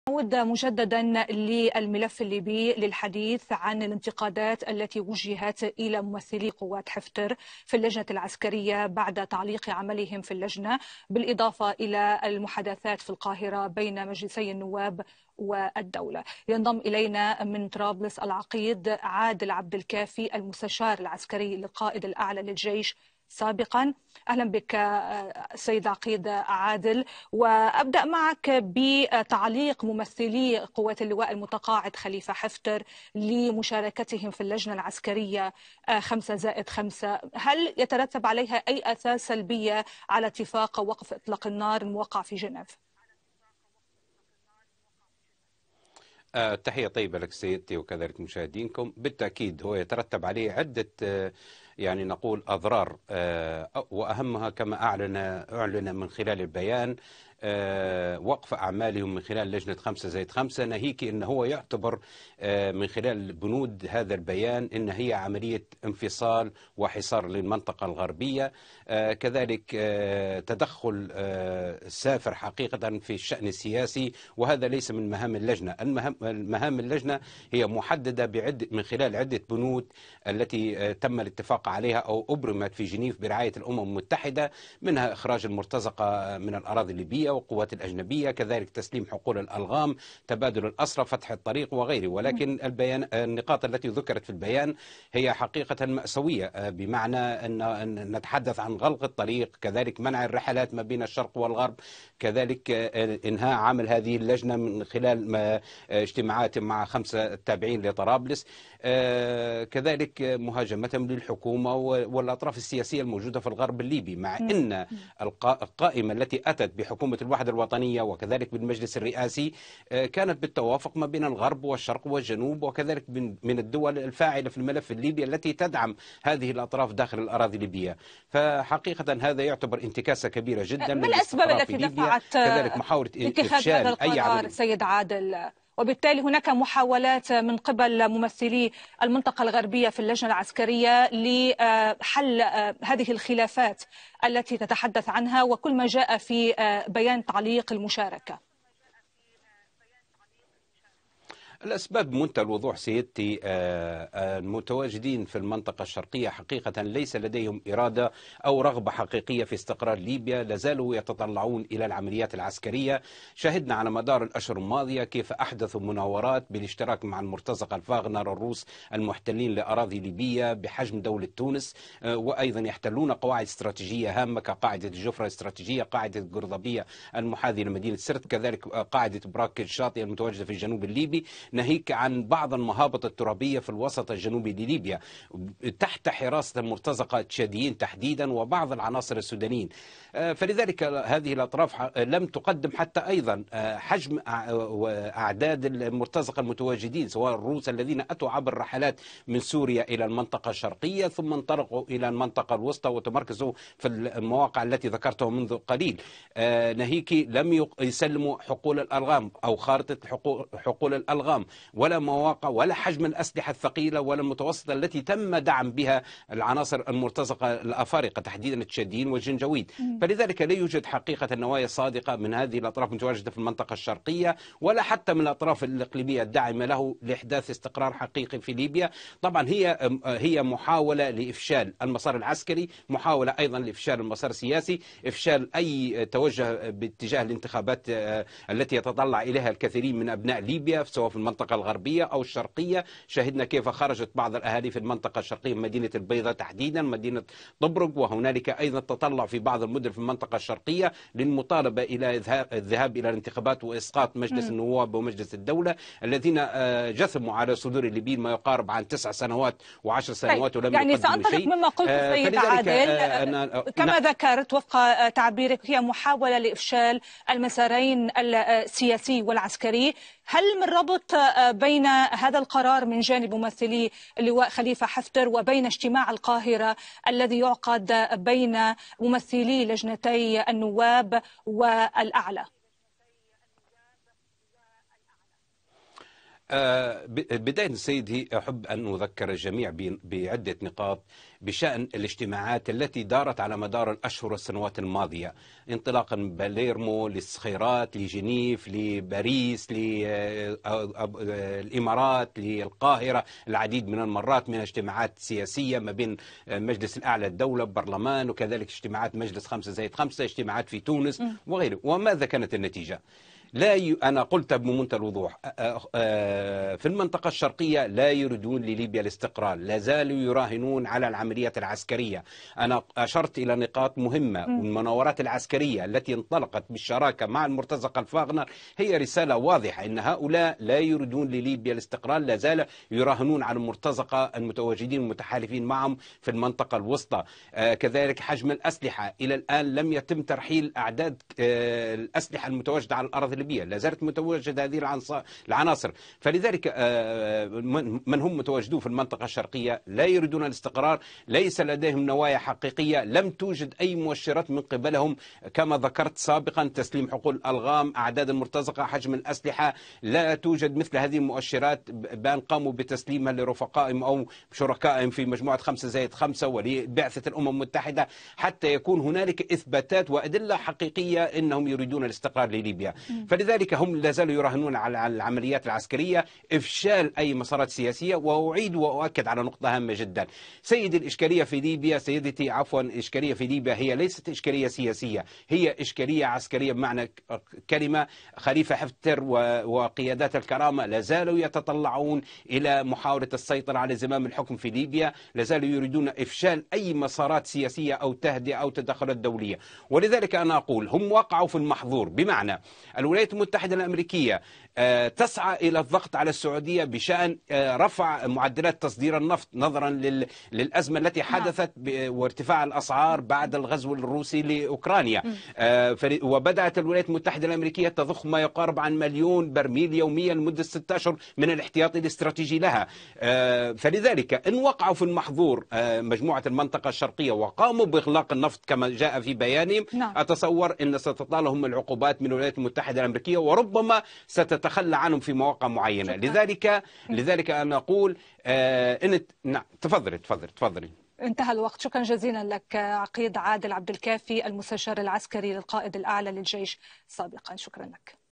ليبيا.. خلافات بين الأطراف السياسية تهدد مسار مفاوضات القاهرة
انتقد ممثلو المنطقة الغربية في اللجنة العسكرية الليبية تعليق نظرائهم من قوات حفتر في اللجنة عملهم، في الوقت الذي تتواصل فيه المحادثات في القاهرة بين نواب من مجلس النواب وأعضاء تابعين لمجلس الدولة.